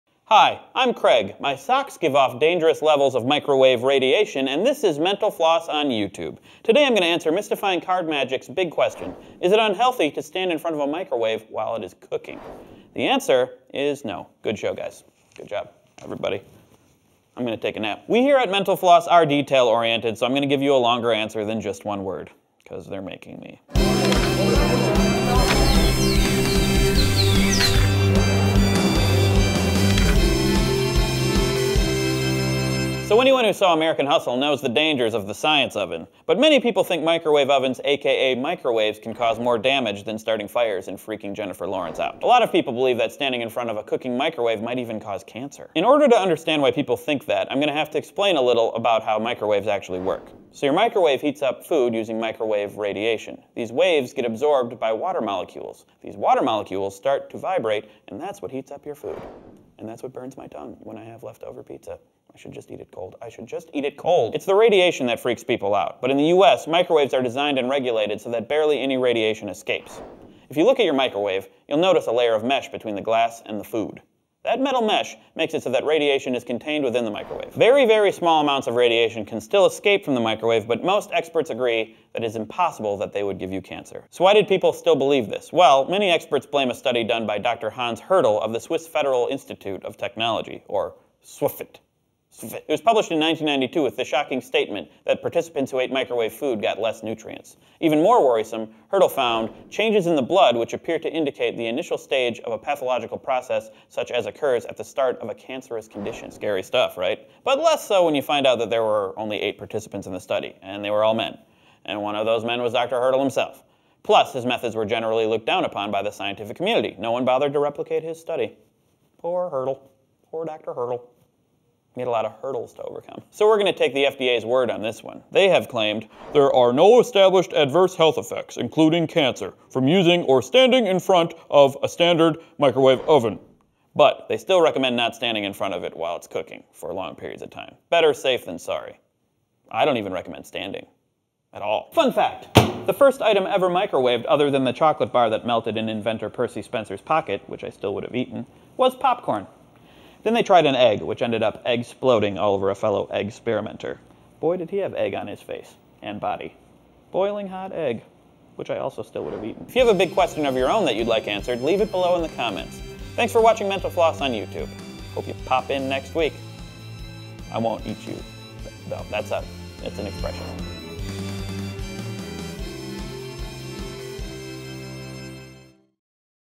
Is it Unhealthy to Stand in Front of a Microwave While it is Cooking? - Big Questions (Ep. 7)
A weekly show where we endeavor to answer one of your big questions. This week, "Mystifying Card Magic" asks: "Is it unhealthy to stand in front of a microwave while it is cooking?"